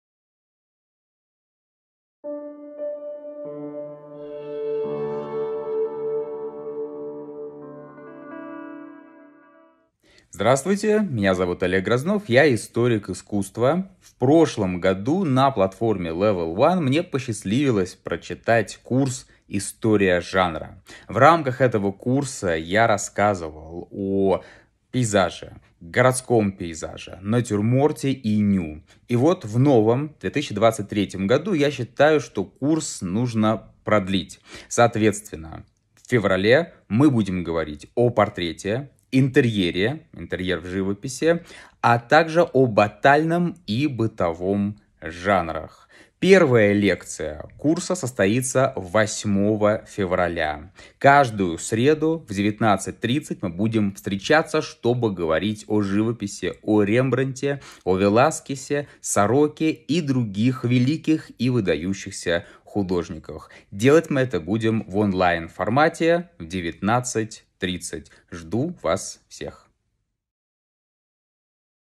Олег Грознов: Онлайн-курс "История жанра"
С 8 февраля (по средам) в 19:30 приглашаем всех на онлайн-курс Олега Грознова "История жанра".

📢 Курс состоит из четырех лекций, посвященных развитию таких жанров мировой живописи, как портрет, интерьер, а также бытовой и батальный жанры. В рамках курса мы поговорим о произведениях великих художников — Рембрандта, Веласкеса, Сороки, Тенирса. Мы обсудим особенности каждого из названных жанров и разберемся в том, какое место они занимают в пространстве культуры.

План курса:
1. История портрета: от королей до Мерилин Монро (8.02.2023)
2. Батальный жанр — картины о войне (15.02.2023)
3. Искусство повседневности: бытовой жанр (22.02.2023)
4. Самый тихий жанр — интерьер в живописи (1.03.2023)

🔎 Лектор - Олег Грознов, научный сотрудник ГБУК «Библиотека-читальня им. И.С. Тургенева», лауреат конкурса «Первая кафедра 2019» в номинации «Гуманитарные науки».